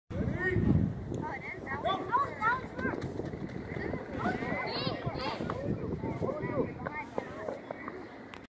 SMBA Hard 90!
At SMBA camp, players learn how to run with proper running mechanics, then we do a hard 90 to see how much they improve in speed and skill running bases!